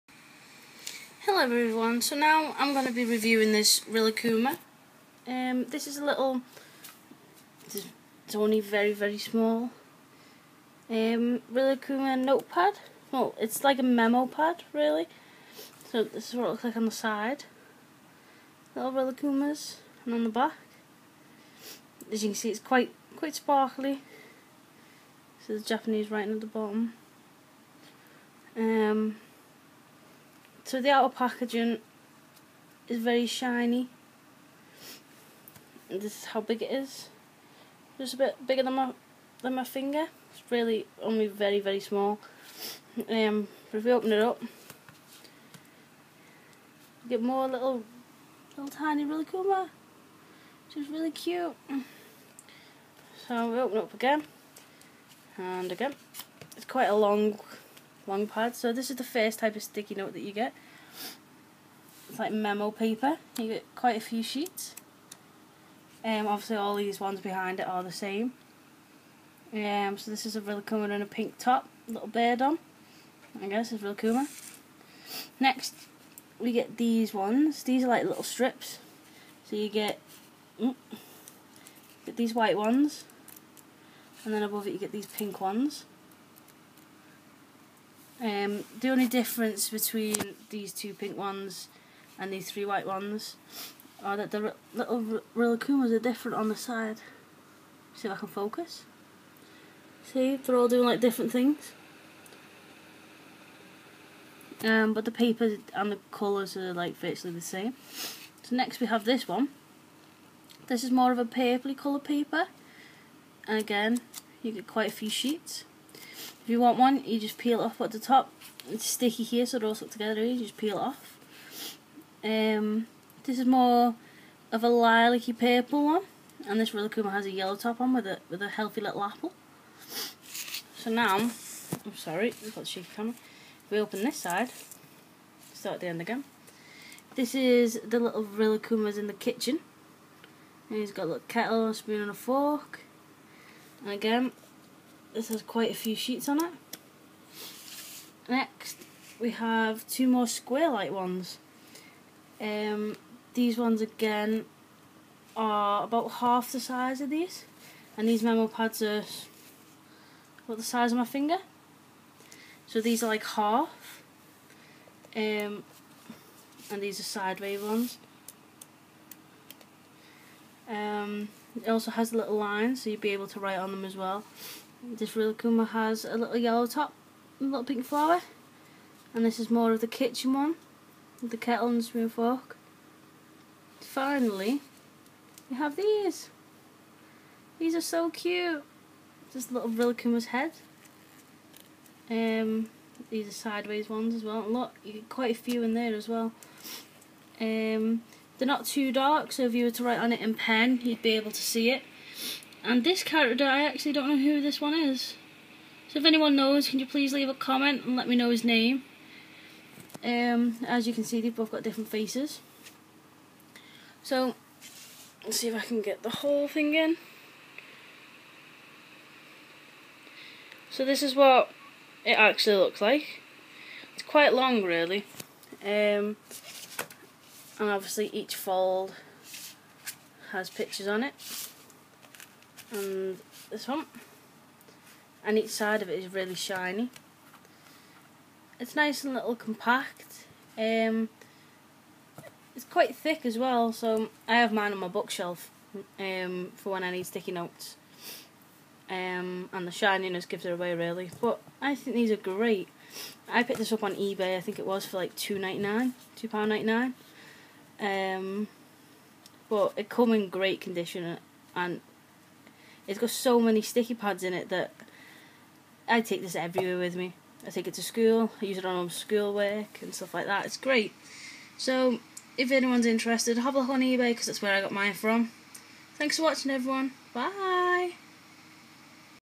Rilakkuma memo pad - Japanese review
another items review from japane and its my rilakkuma memo pad ^_^ this memo pad it only small but has many stickers in it as you will see in my video! hope you like my item and please leave a video responce if you have items that are simillar